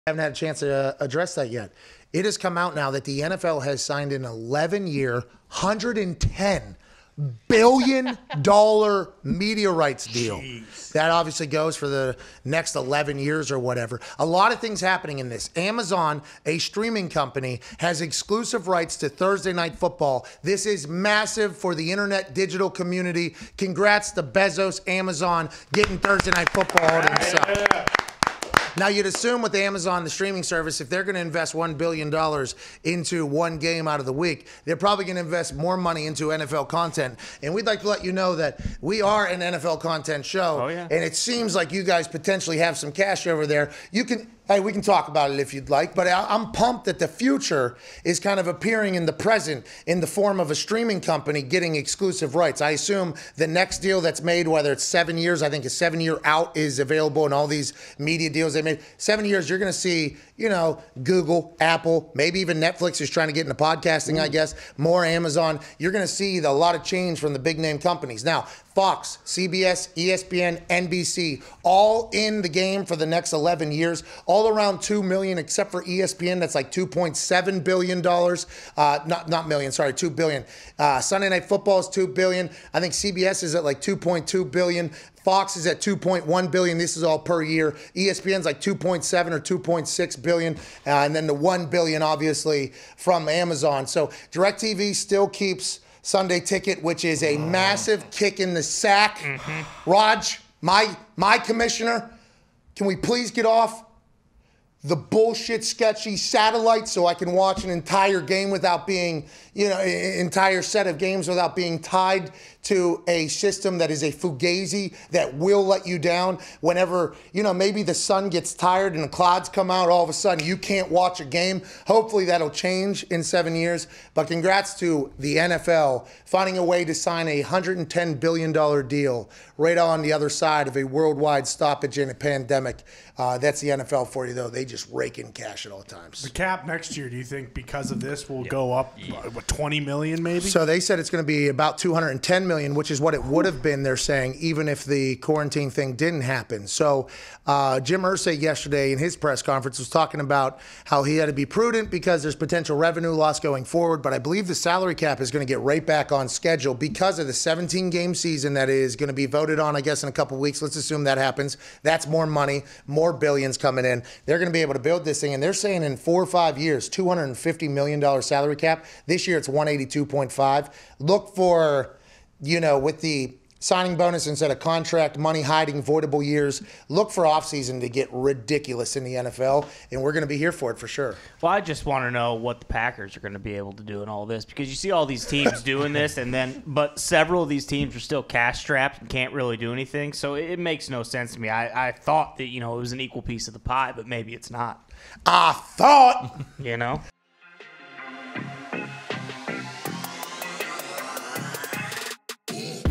Pat McAfee Reacts To The NFL's $110 BILLION TV Deal
Somehow the NFL always finds a way to print money..